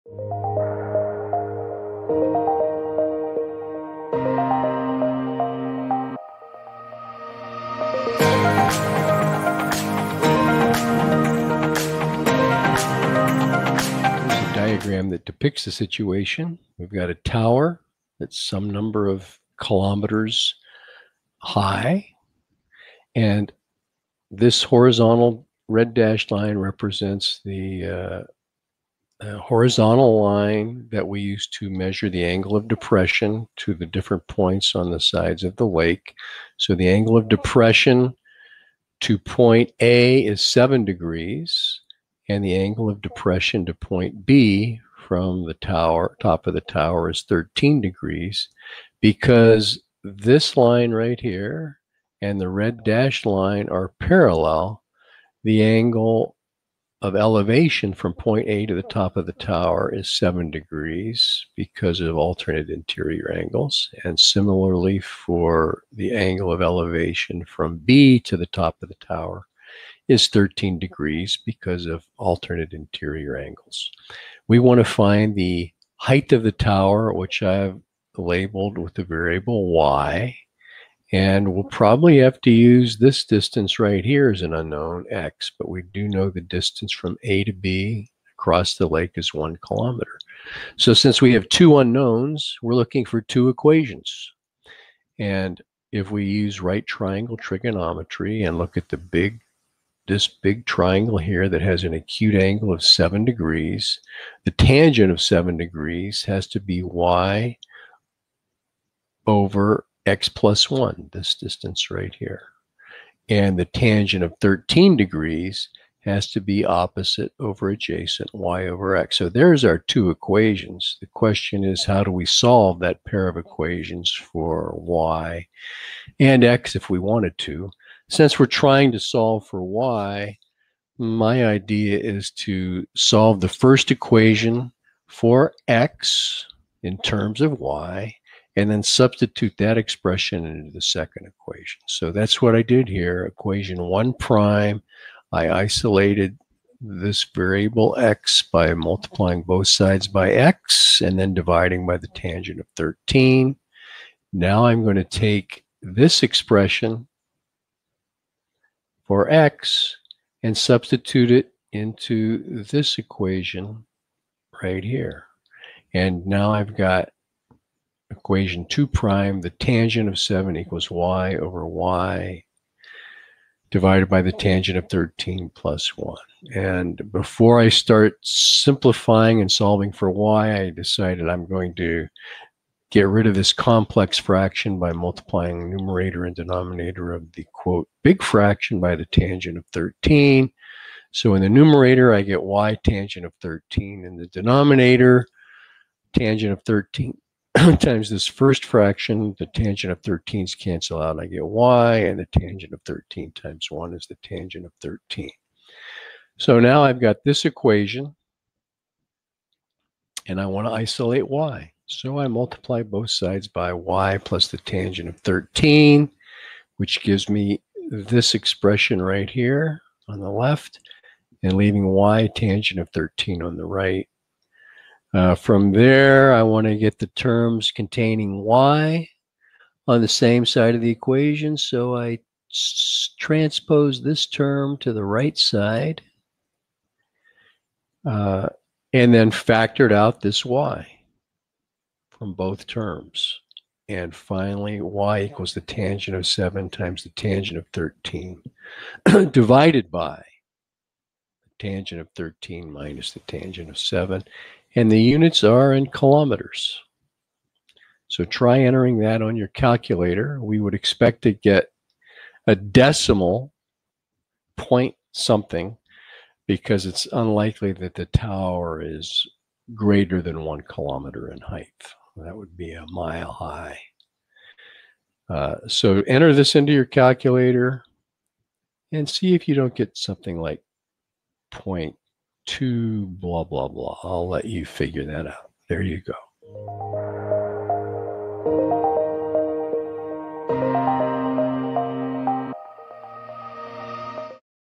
Find the height of a tower given angles of depression to points on opposite sides of a lake.
Question: Help me with this trigonometry/problem solving question.
From an observation tower that overlooks a small lake, the angles of depression of point A, on one side of the lake, and point B, on the opposite side of the lake are, 7° and 13°, respectively, the points and the tower are in the same vertical plane and the distance from A to B is 1km. Determine the height of the tower.

Answered By:

Doug C.

Written Explanation:

Using Law of Sines is also an option.If h is the hypotenuse of the triangle with an acute angle of 13 degrees we can use the triangle with a side of 1 KM and angles of 7 and 6 degrees to reach: h/sin7 = 1/sin 6. That is, h = sin 7/sin 6.Then from the right triangle with acute angle 13 degrees we have sin 13 = y/h. So the height of the tower can also be found as y = (sin7)(sin13)/(sin6)